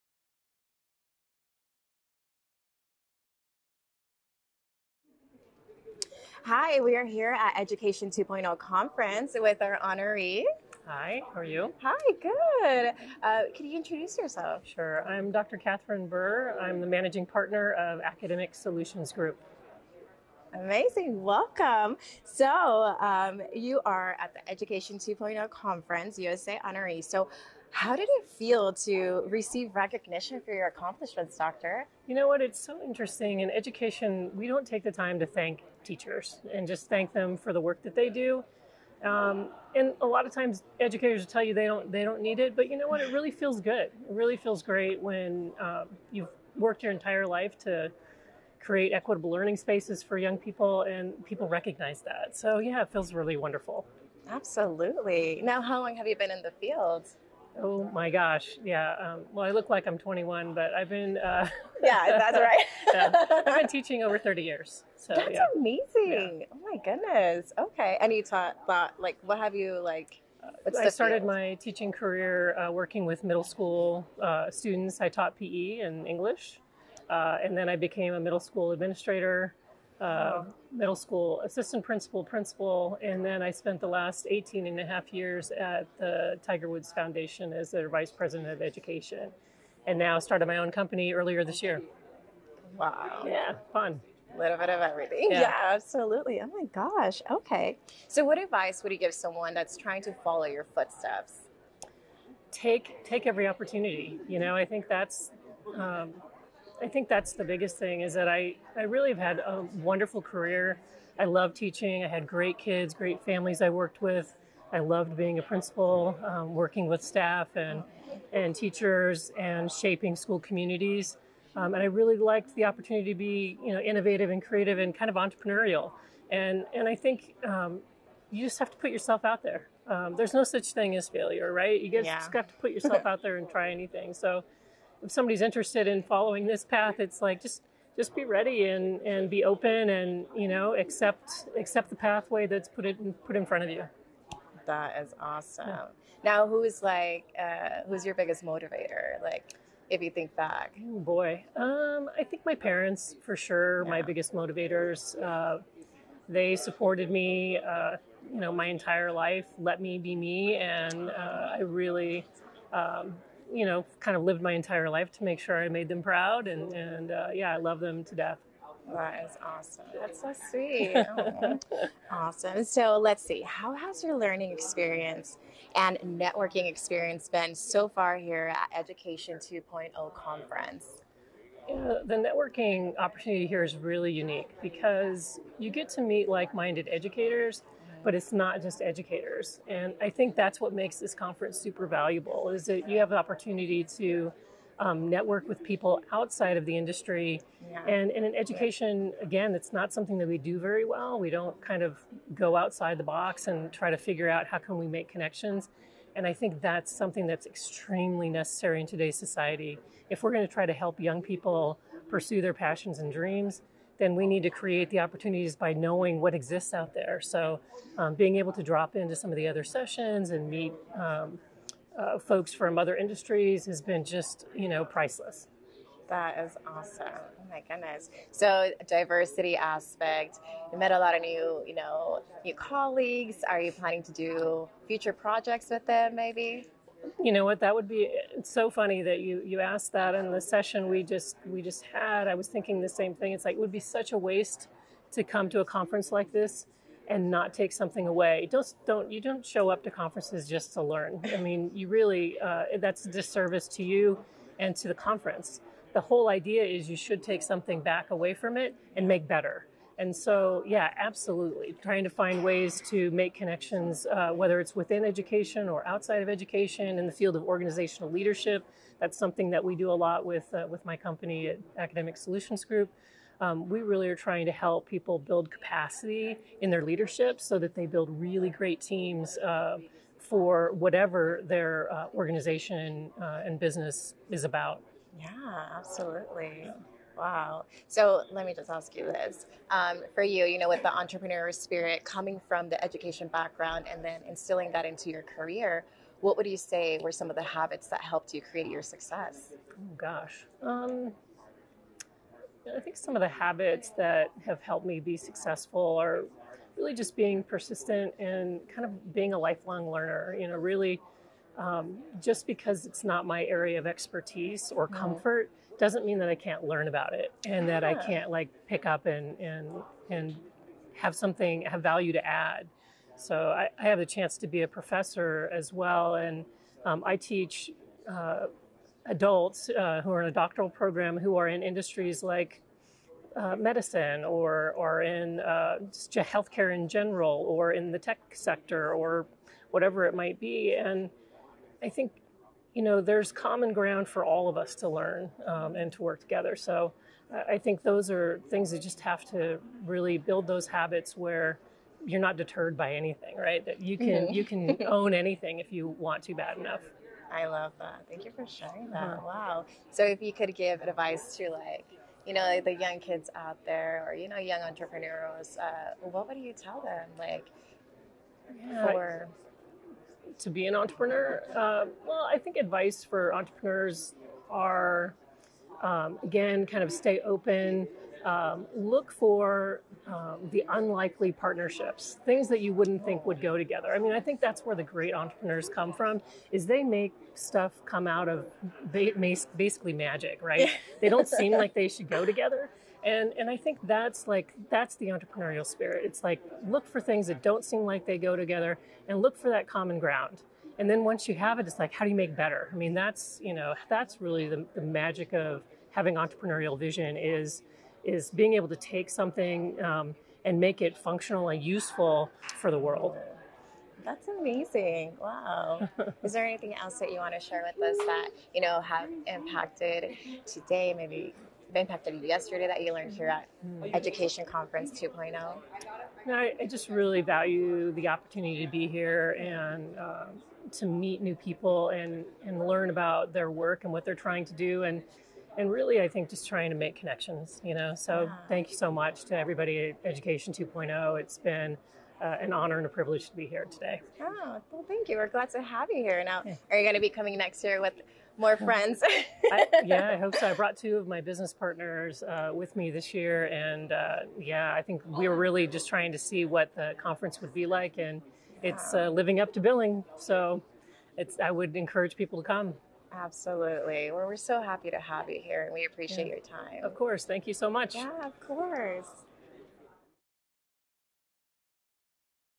Katherine Bihr | Outstanding Leadership Award
Katherine Bihr, Managing Partner of Academic Solutions Group , was honored with the Outstanding Leadership Award at the Winter Edition of the Education 2.0 Conference, which took place at Mandalay Bay, Las Vegas, USA, on December 19-21, 2022. In this interview, she enthusiastically shared her experience at the three-day event and reflected on being recognized on a global platform.